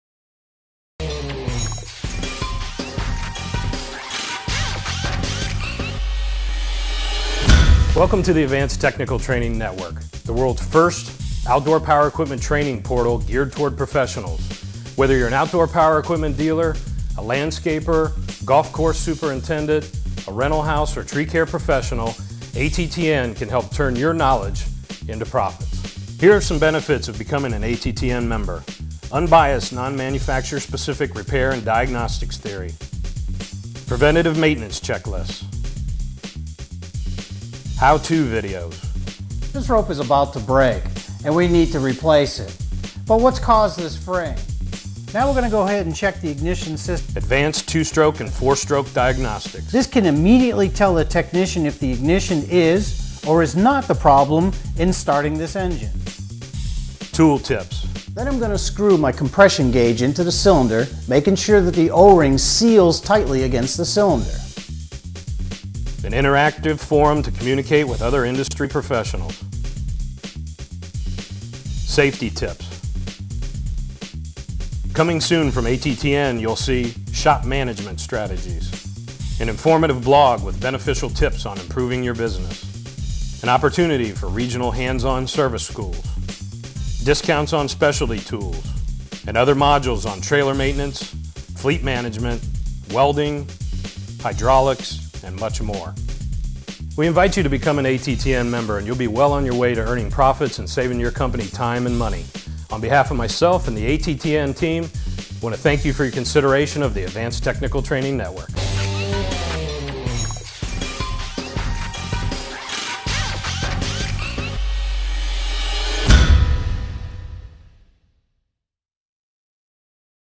What is ATTN?
Advanced Technical Training Network (ATTN) is the world's first ope equipment training portal for professionals